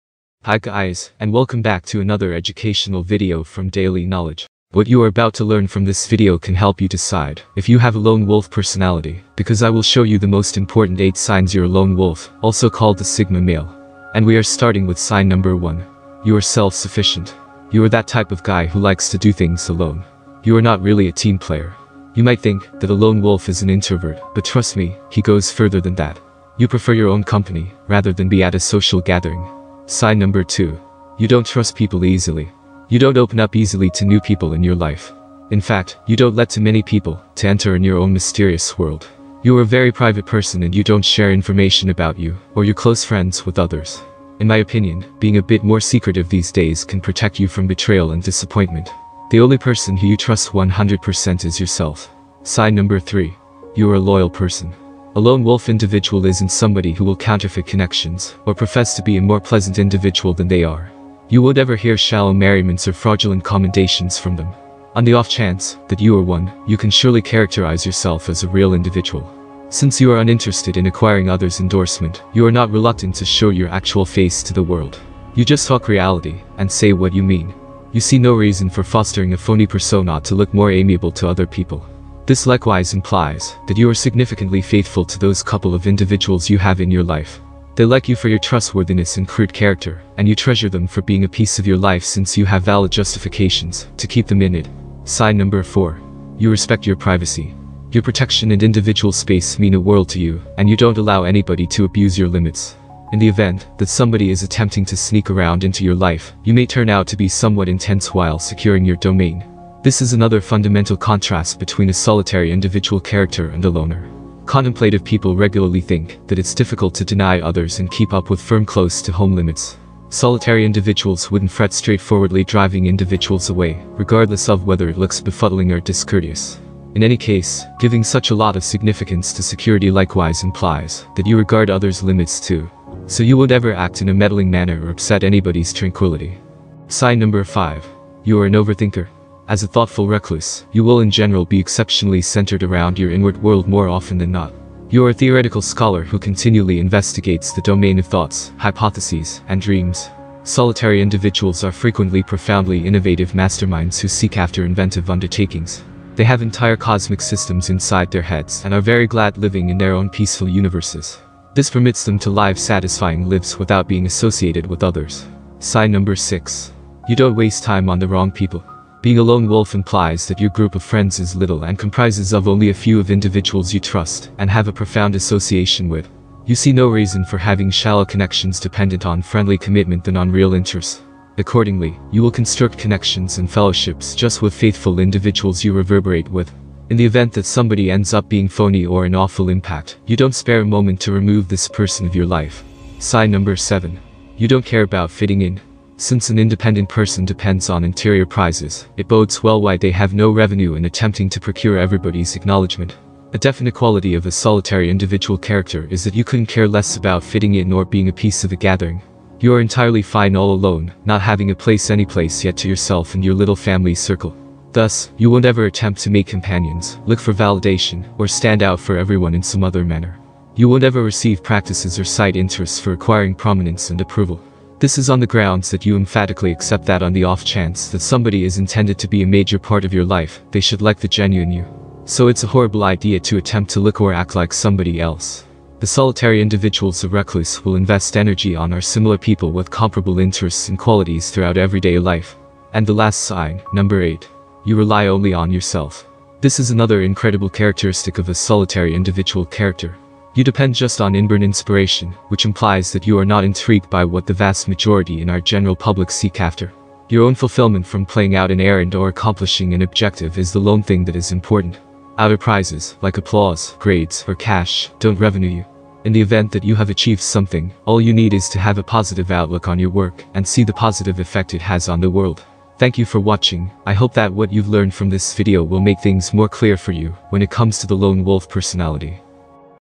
8 Signs You're a Lone Wolf The Sigma Male
8 Signs You're a Lone Wolf The Sigma Male

Hi everyone,  what you're about to learn from this educational video can help you decide if you have a LONE WOLF personality also called THE SIGMA MALE. SIGMA MALE IS ONE OF MY FAVORITE TOPIC WHEN IT COMES TO MALE PERSONALITIES SO I MADE A FEW VIDEOS ABOUT IT, IF YOU GO AND CHECK THEM OUT.